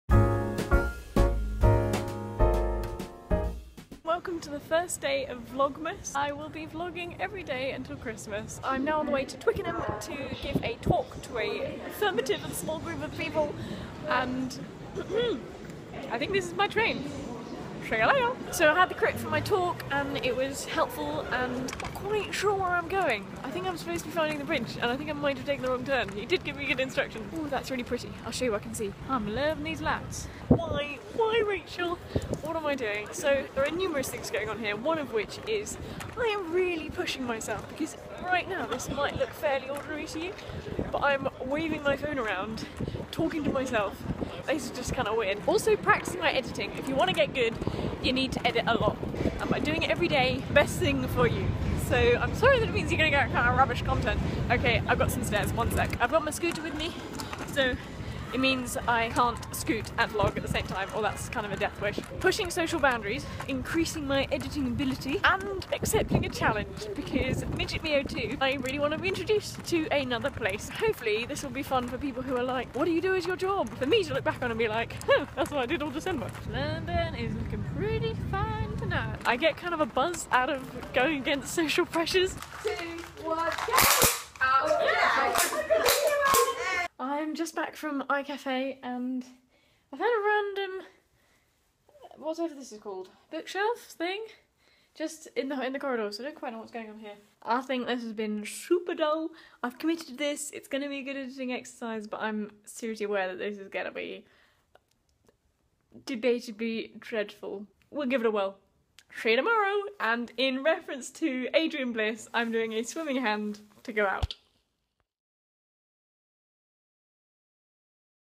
Why?! | Rachel's Vlogmas | 01
I was challenged to make a video everyday up to Christmas. So this is what it feels like to be a daily vlogger?! Here's the 'why' to kick us off!! @rachelmshannon

Channel's referenced are (swimming hand while meta-vlogging) and the inspiration for this challenge and wonderful human (midgetme02)